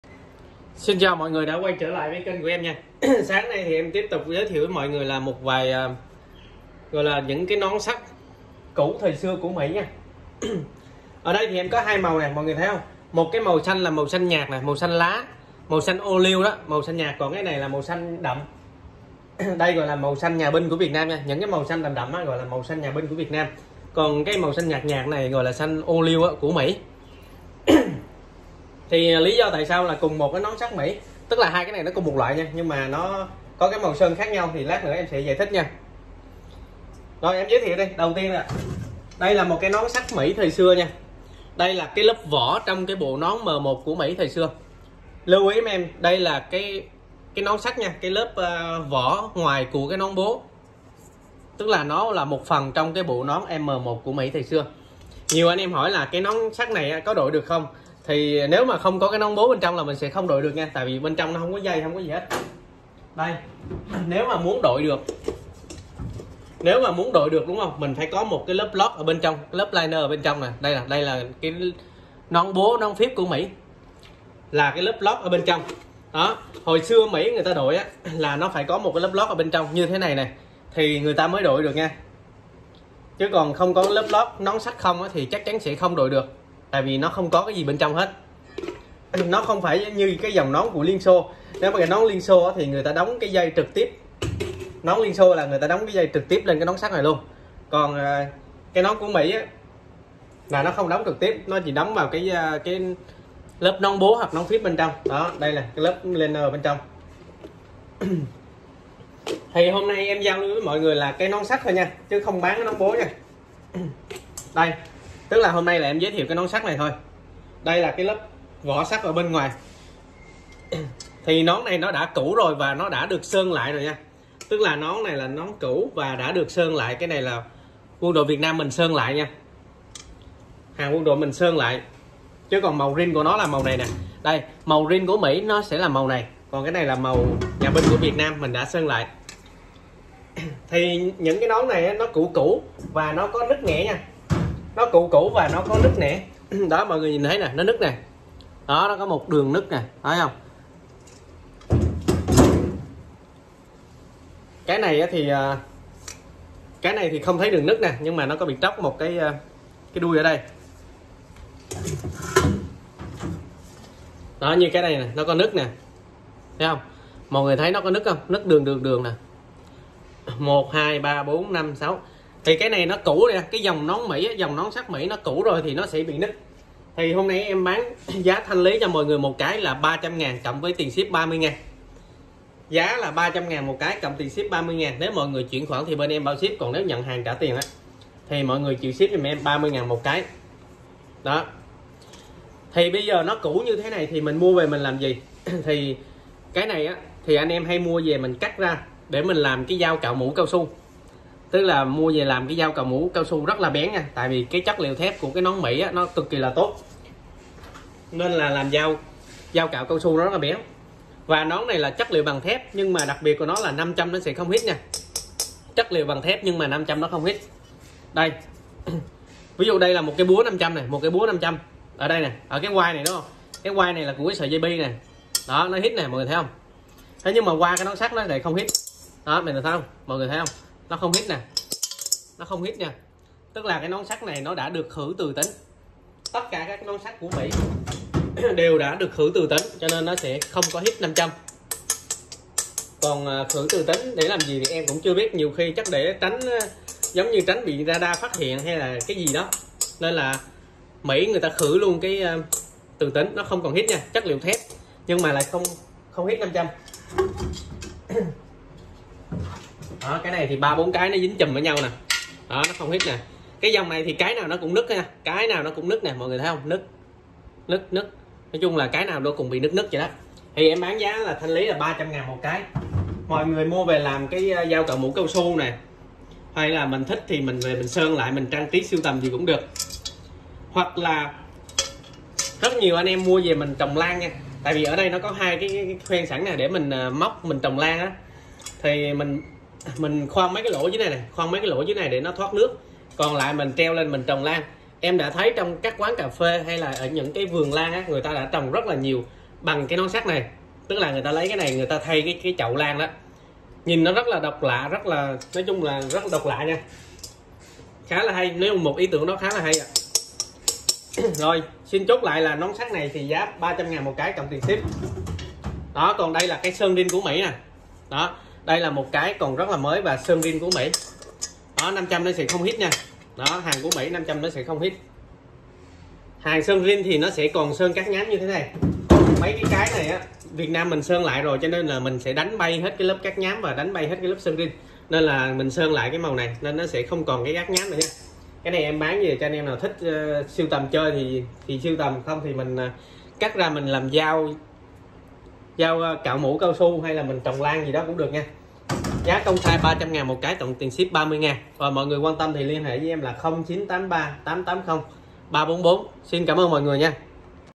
Nón sắt Mỹ xưa giá ve chai, nón M1 US 0983880344
Thanh lý nón sắt Mỹ giá ve chai cực rẻ
Nón sắt là lớp ngoài nón bố trong bộ M1 US xưa
Nón đã được sơn lại, cũ theo thời gian, có vết rạn nứt
Sử dụng làm dao cạo, chậu trồng lan, sưu tầm đều được
Giá rẻ như ve chai